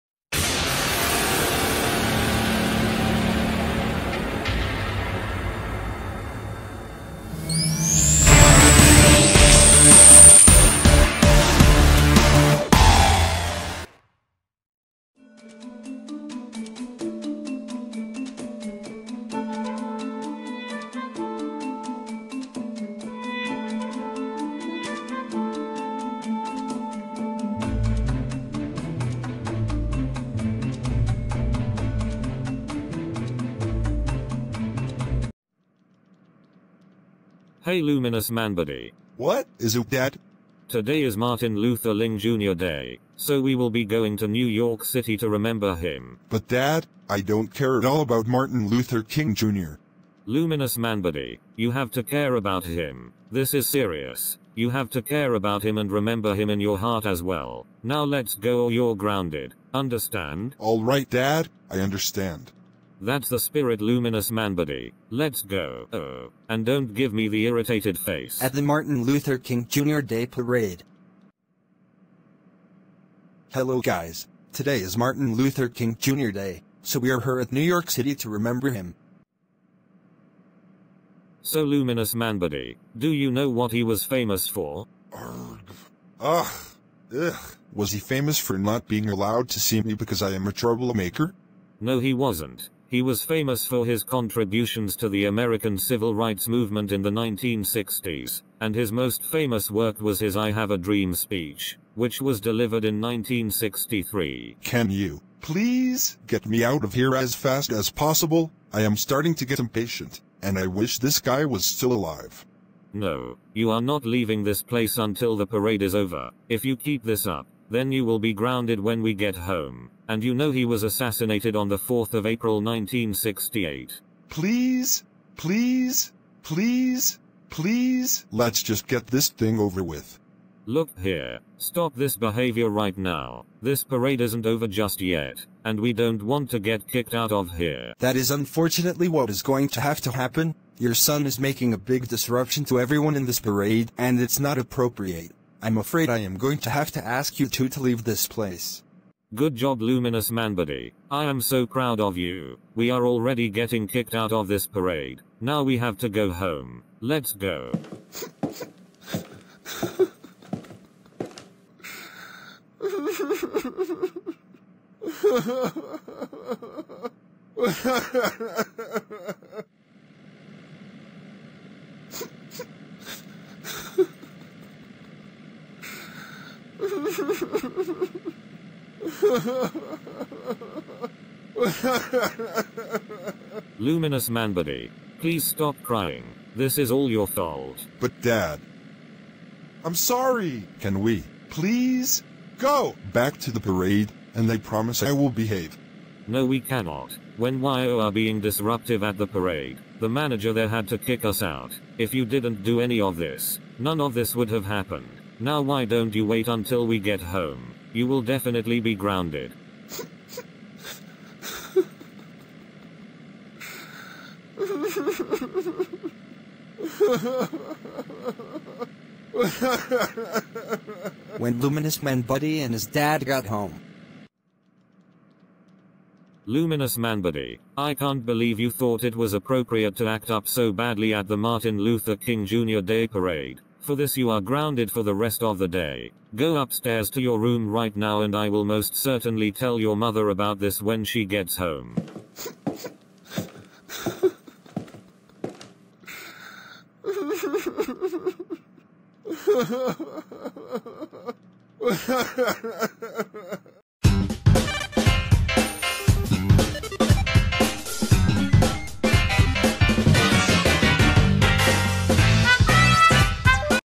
Luminous Manbuddy Gets Grounded On Dr Martin Luther King Jr Day
This video is a special for Dr Martin Luther King Jr Day,

Happy Dr Martin Luther King Jr Day

This video was also requested by @user-Natsu2001